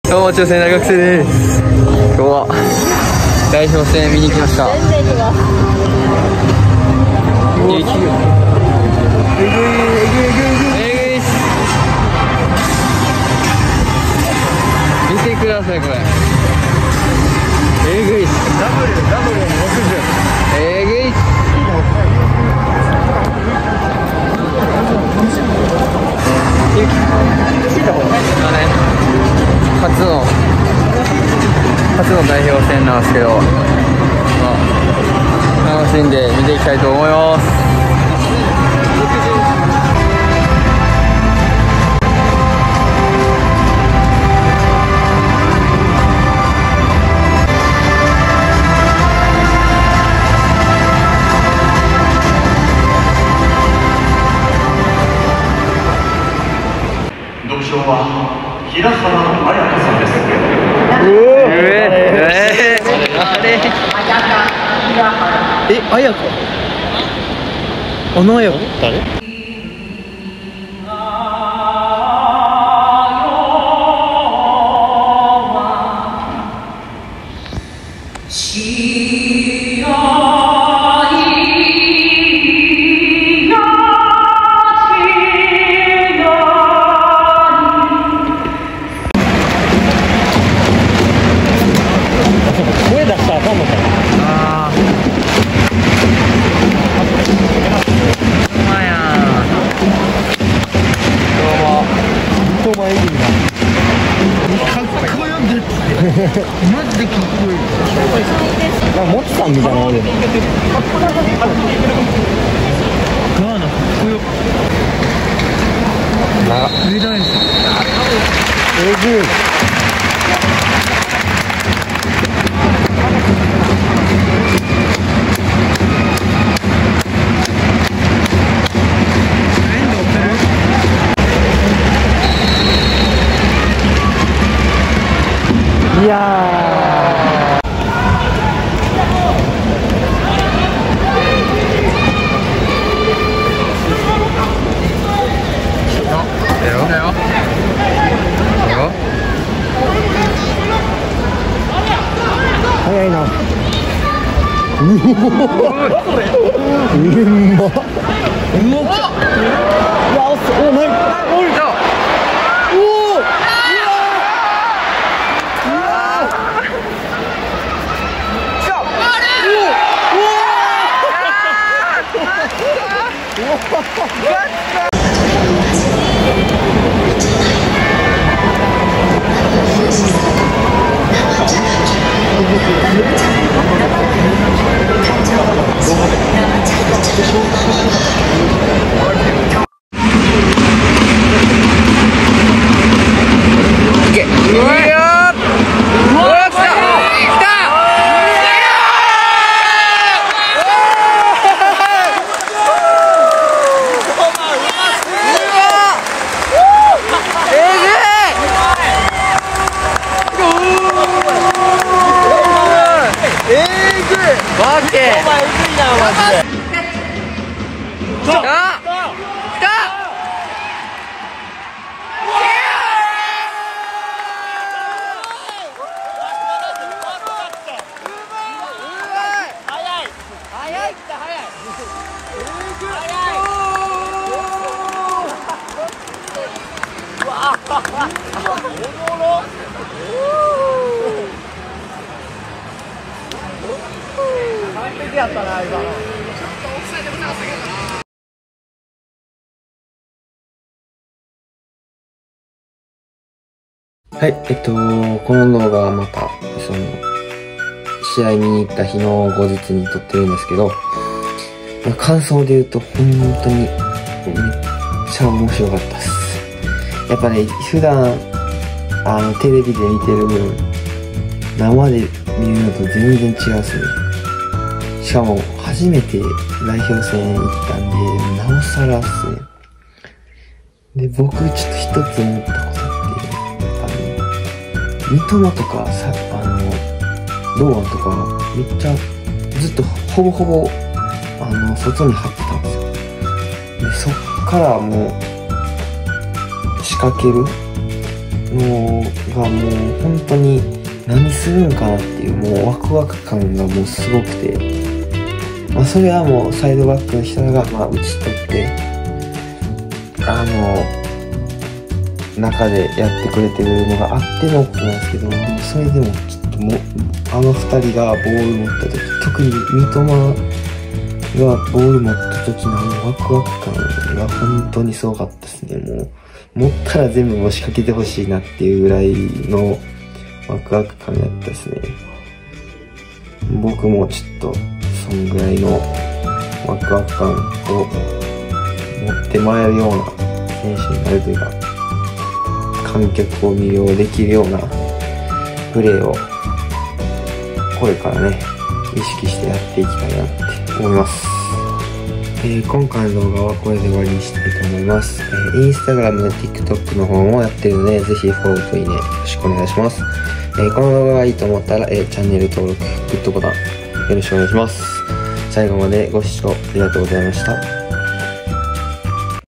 【vlog】人生初めての代表観戦！！三苫と堂安がボール持った時のワクワク感と高揚感が漂うあの雰囲気がヤバいwww
人生で初めて代表戦を観に行きました！あのワクワクとか緊張感はテレビで見るのと全然違いました！
特に三苫と堂安の観客を魅了するような雰囲気はマジで凄かったです！